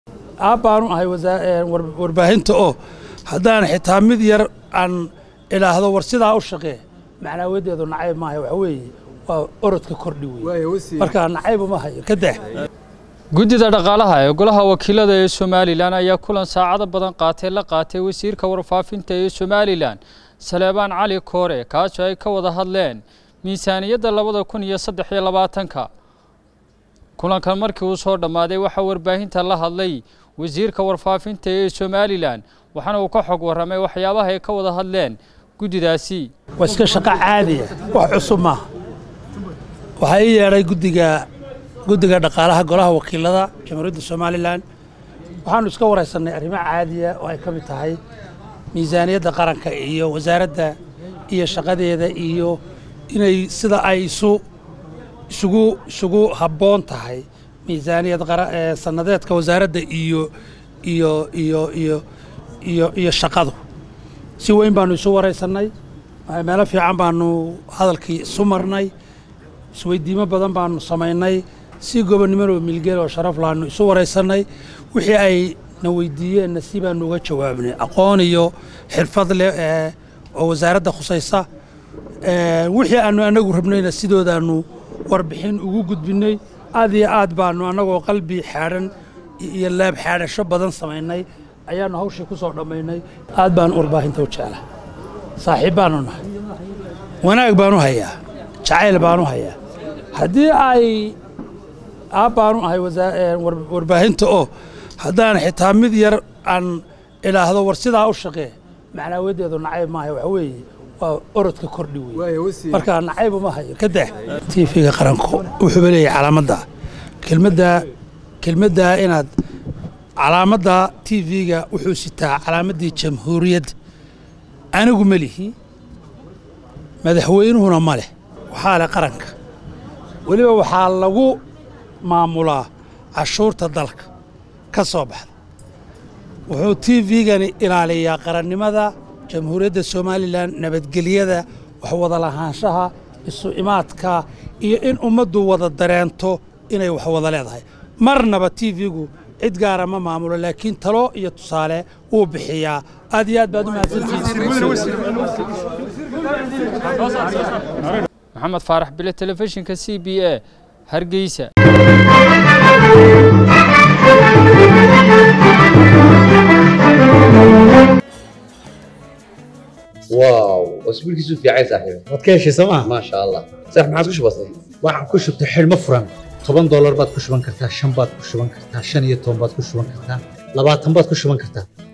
Xukuumadda oo ka jawaabtay faaladda Telefishanka qaranka ka baxda in lagu caayo madaxda mucaaradka.
Xukuumadda Somaliland ayaa ka jawaabtay faaladda Telefishanka qaranka ka baxda in lagu caayo madaxda mucaaradka